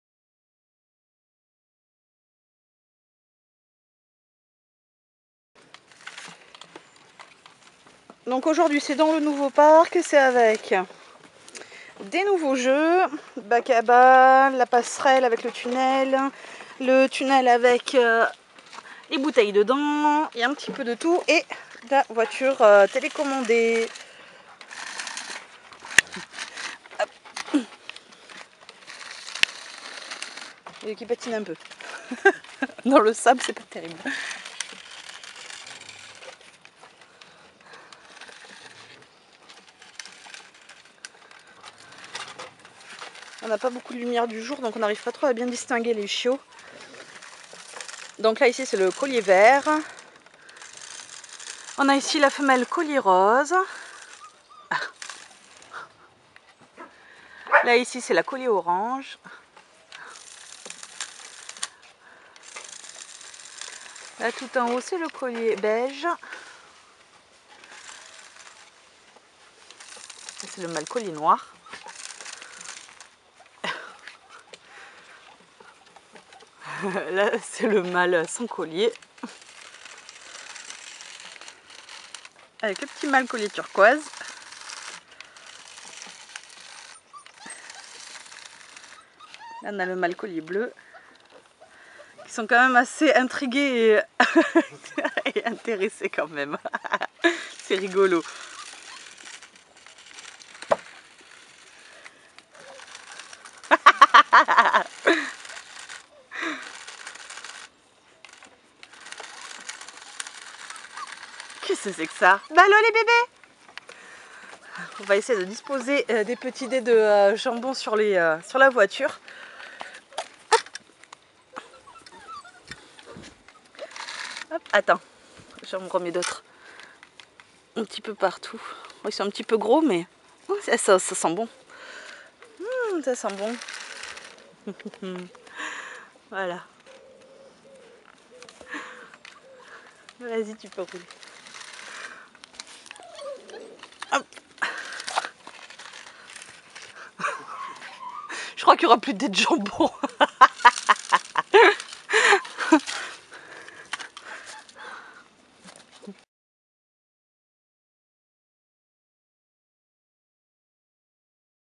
Chiots Berger Malinois âgés de 40 jours - Elevage Gaia Vlka
Sociabilisation à la voiture télécommandé